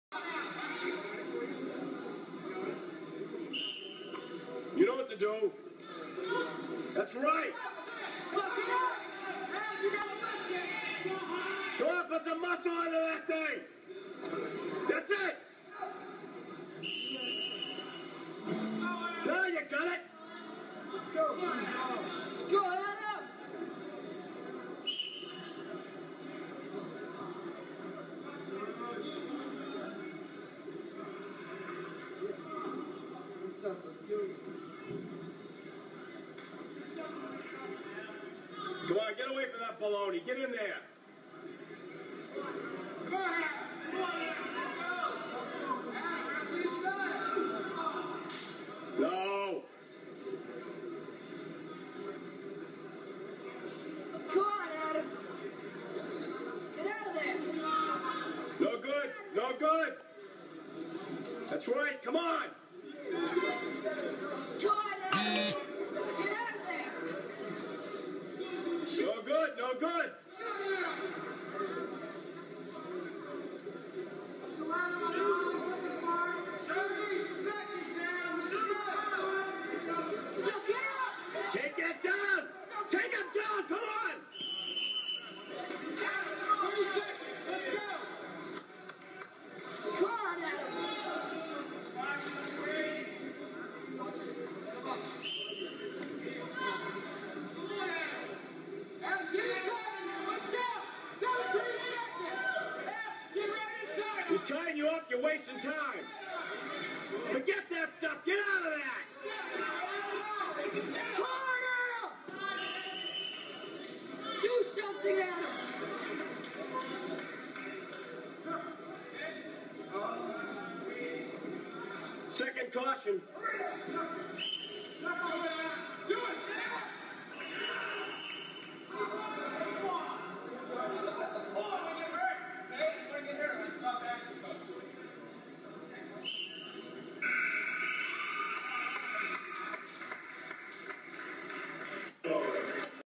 Wilmington invitational 01.01.1992
Adam Goujon giving a great match in the finals